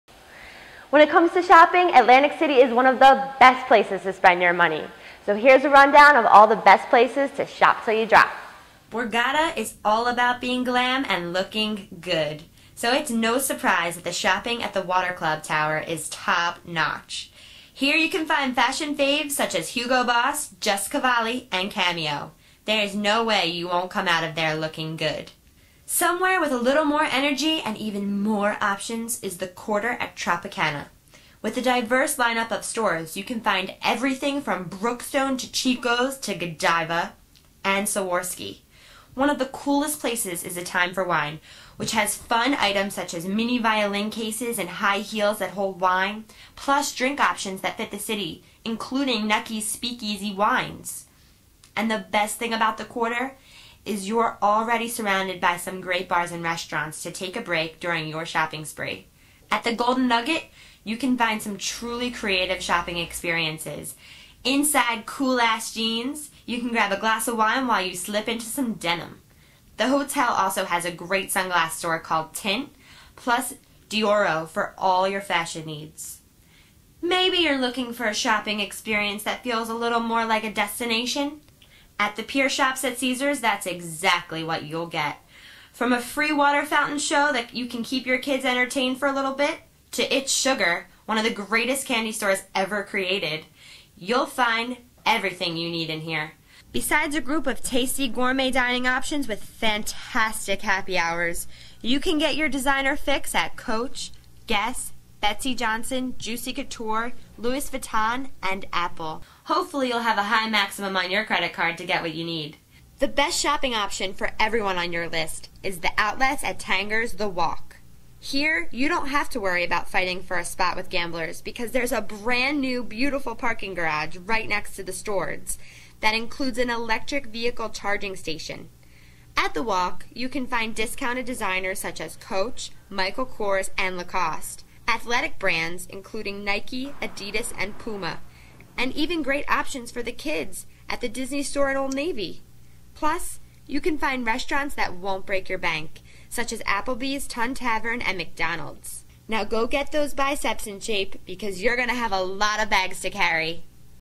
The Best Shopping in Atlantic City
Atlantic City has great shopping centers, including; Tanger Outlets The Walk, The Quarter at Tropicana, The Pier Shops at Caesars, Golden Nugget, and The Water Tower at Borgata.  Video by Michael Ein